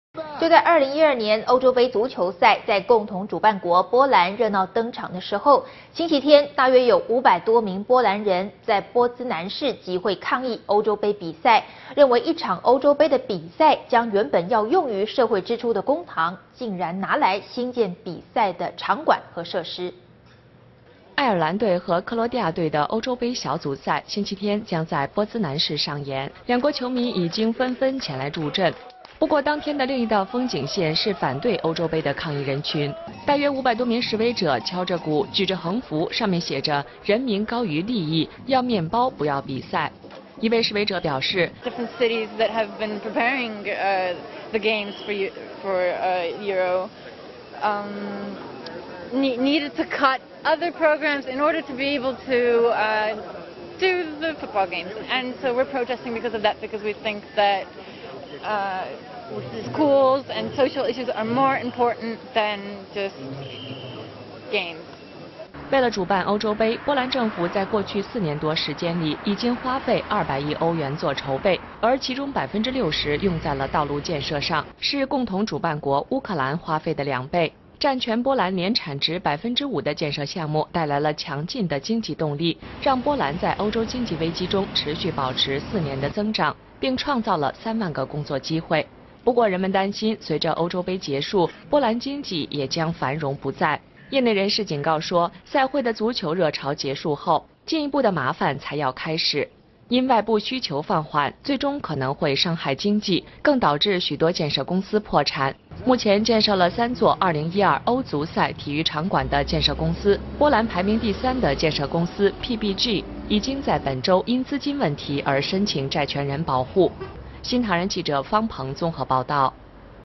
波蘭民眾示威 抗議歐洲盃花費
【新唐人亞太台2012年6月11日訊】就在2012年歐洲盃足球賽，在共同主辦國波蘭熱鬧登場的時候，星期天(10號)大約有500多名的波蘭人，在波茲南市集會抗議歐洲盃比賽，認為一場歐洲盃的比賽將原本要用於社會支出的公帑，竟然拿來興建比賽的場館和設施。

愛爾蘭隊和克羅埃西亞隊的歐洲盃小組賽星期天將在波茲南市上演，兩國球迷們已紛紛前來助陣。

不過當天的另一道風景線是反對歐洲盃的抗議人群。大約500多名示威者敲著鼓，舉著橫幅上面寫「人民高於利益」、「要麵包不要比賽」。

抗議民眾：「為了支應這場足球比賽，其它城市因而必須減少其他計劃支出。但我們認為，學校和社會的經費，比足球比賽更重要。」

為了主辦歐洲盃，波蘭政府在過去四年多時間裡已經花費200億歐元做籌備，而其中60%用在了道路建設上，是共同主辦國烏克蘭花費的兩倍。

佔全波蘭年產值百分之五的建設專案帶來了強勁的經濟動力，讓波蘭在歐洲經濟危機中持續保持了4年的增長，並創造了 3萬個工作機會。不過人們擔心隨著歐洲盃結束，波蘭經濟也將繁榮不再。

業內人士警告說，賽會的足球熱潮結束後，進一步的麻煩才要開始。因外部需求放緩，最終可能會傷害經濟，更導致許多建設公司破產。日前建設了三座2012歐足賽體育場館的建設公司、波蘭排名第三的建設公司PBG，已經在本週因資金問題而申請債權人保護。

新唐人亞太電視 方彭 編譯

其他推薦:
看動態網：曉天下事